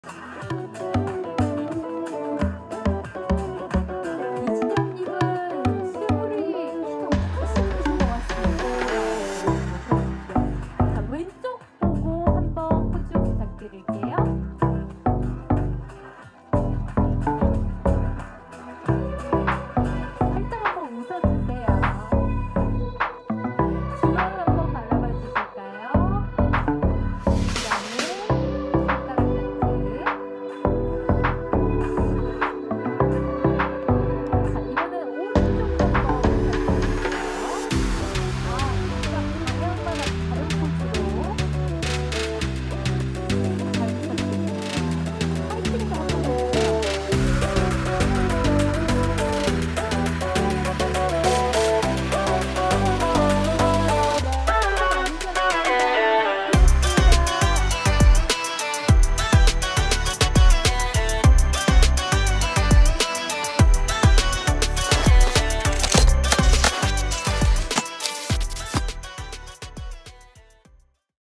[TOP영상] 이진혁(Lee Jin-Hyuk), 비주얼이 명품인 이진혁의 미모(220919 ‘늑대사냥’ VIP시사회)
9월 19일 오후 용산구 CGV 용산아이파크몰에서 영화 ‘늑대사냥(Project Wolf Hunting)’ VIP시사회가 열렸다.

이날 영화 '늑대사냥’ VIP시사회에서 서인국-장동윤-박호산-정소민-고창석-장영남이 참석했으며 수많은 인기스타들이 참석했다.

'늑대사냥'은 극악무도한 범죄자들을 태평양에서 한국까지 이송해야 하는 상황, 극한 상황에 처하게 되는 이들의 이야기를 하드보일드 서바이벌 액션 영화다.